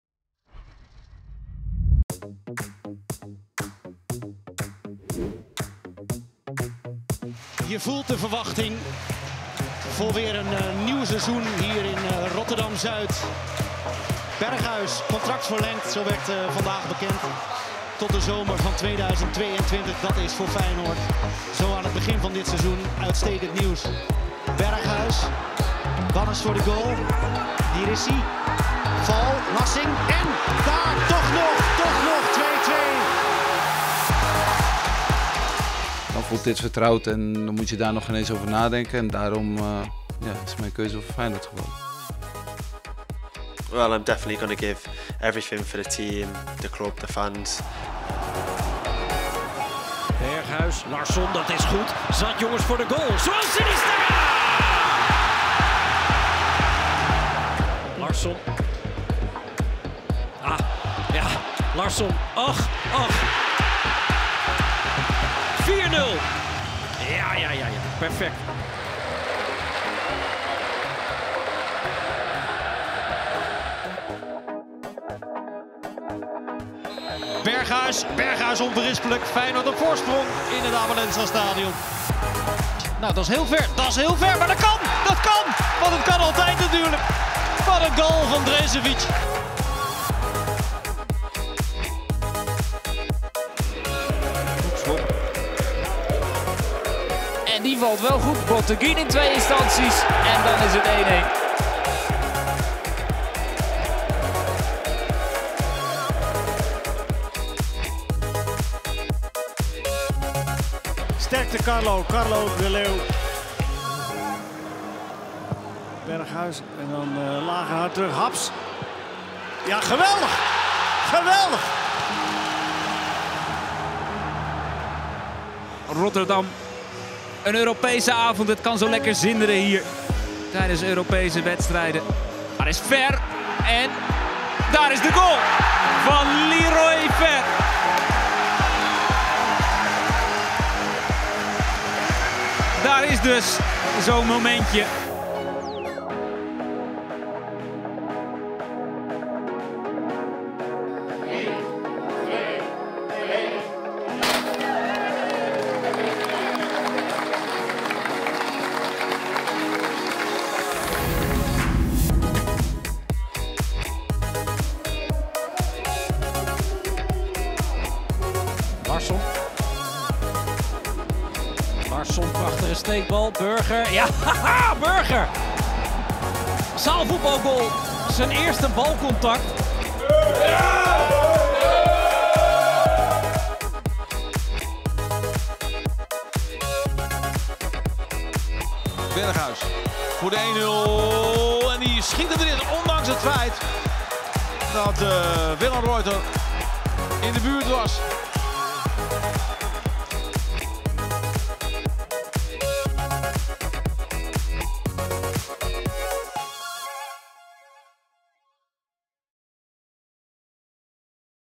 Highlights | Zo verliep de eerste maand van seizoen 2019-2020
De eerste maand van het nieuwe voetbalseizoen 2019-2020 zit erop. Kun jij ook niet wachten tot de Eredivisie en Europa League weer begint?